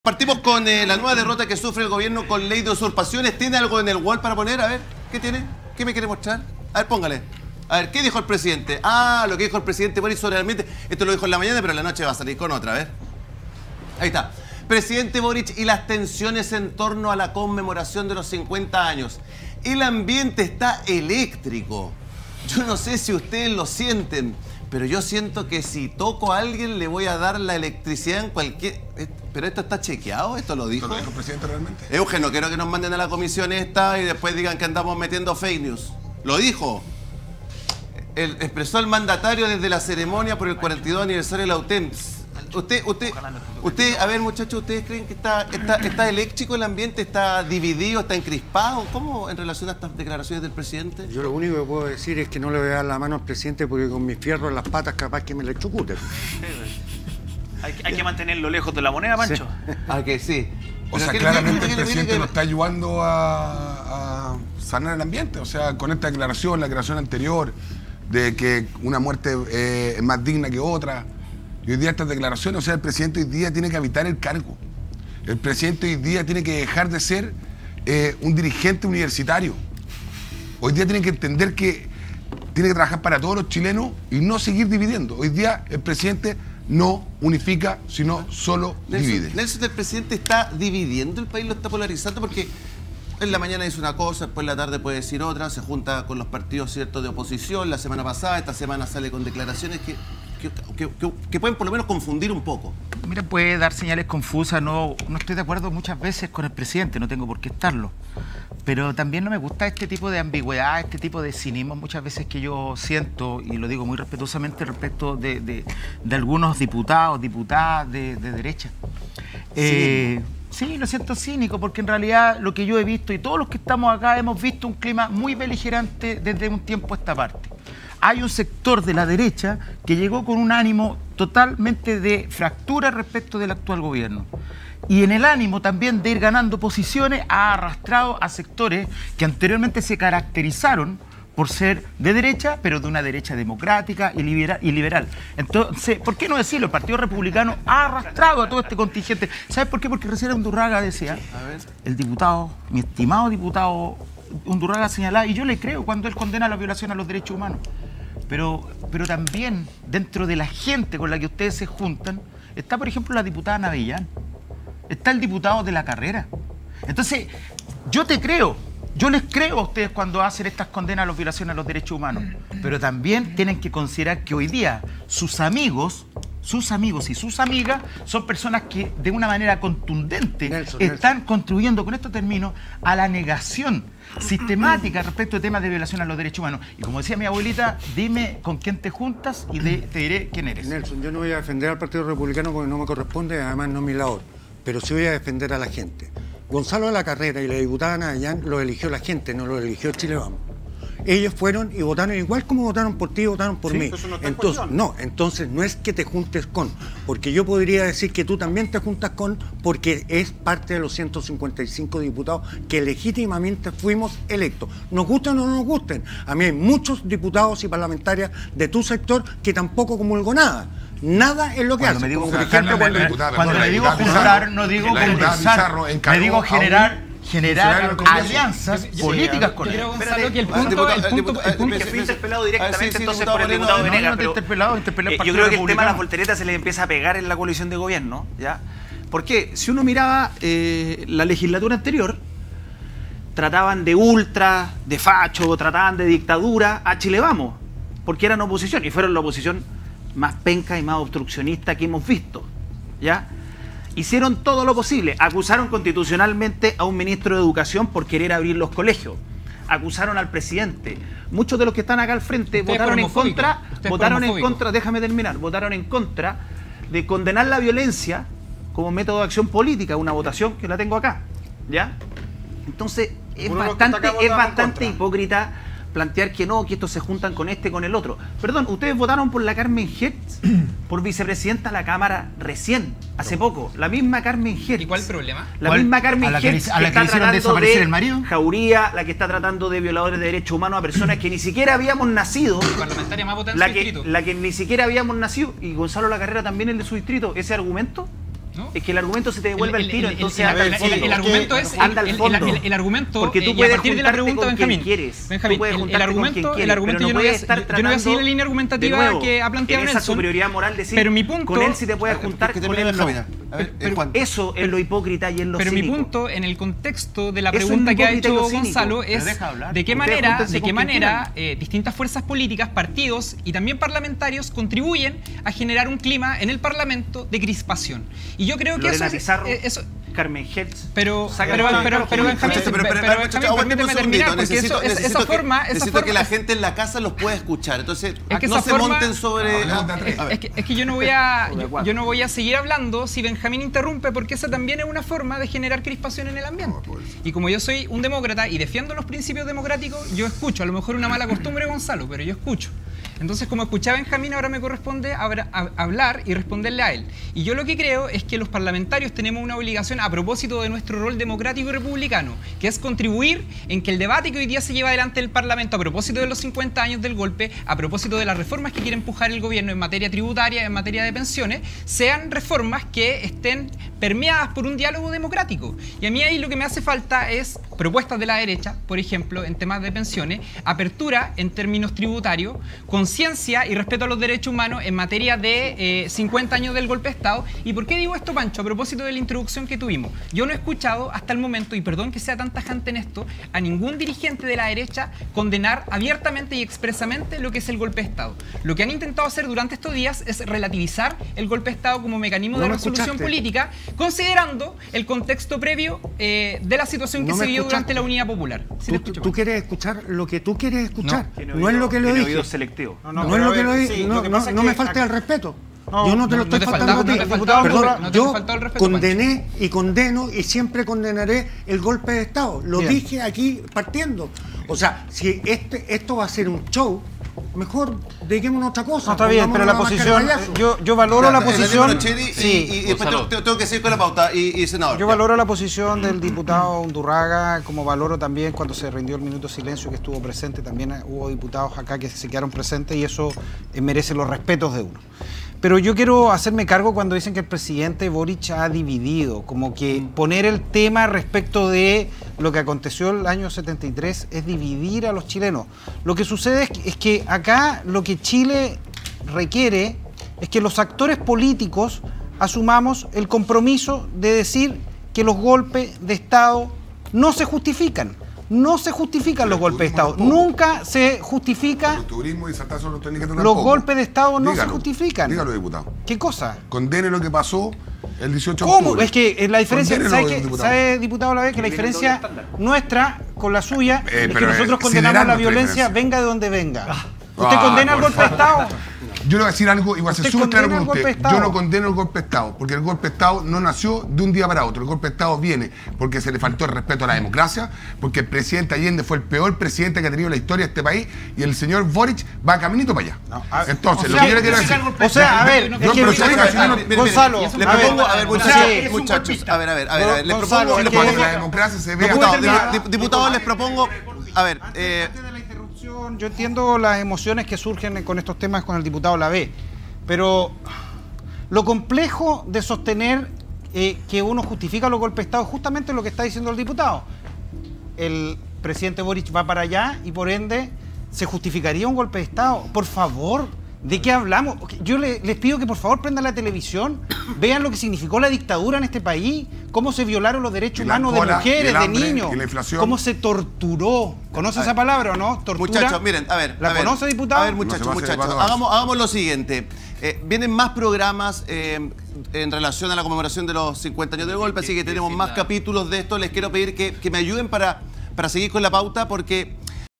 BORIC ELECTRICO con MIEDO de ELECTROCUTAR a QUIÉN SE LE ACERQUE
¡Alta tensión! 🔥 En el episodio de hoy los panelistas: Cristian Labbe, Nelson Venegas, Benjamin Moreno, Daniel Manouchehri, Guillermo Ramirez, Juan Santana, Francisco Undurraga y Karim Bianchi analizan y debaten la contingencia. 🇨🇱 Prepárate para un acalorado cruce de ideas.

¿Estás listo/a para un debate con alta tensión? 🥊 Nuestros panelistas desgranan los temas más importantes del día: En el aniversario de la UTEM, el presidente Boric habló de la tensión entre oficialismo y oposición debido a la conmemoración del golpe de Estado. Describió el ambiente como "eléctrico" y expresó su temor de "electrocutar" al tocar a alguien. Destacó la importancia de la memoria y la verdad en los 50 años del quiebre democrático y abogó por sanar heridas a través de políticas públicas como el Plan de Búsqueda.